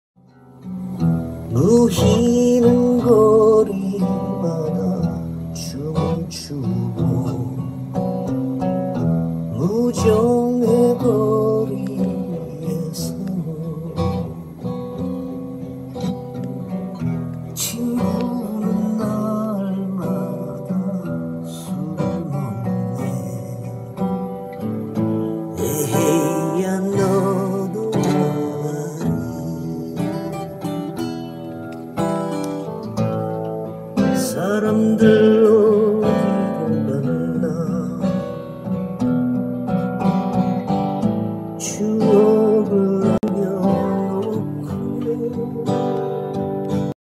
DAT digital transfer to Pyramix
DAT tape is being transferred to the Pyramix using AES3.